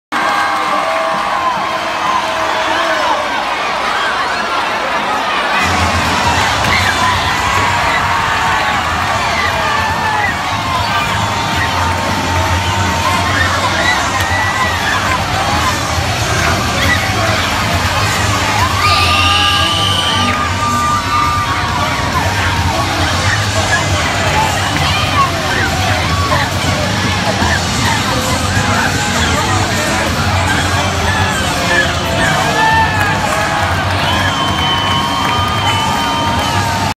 Hockey fight in ECHL game
Hockey fight at Florida Everblades game vs. Jacksonville Icemen, Dec. 8, 2017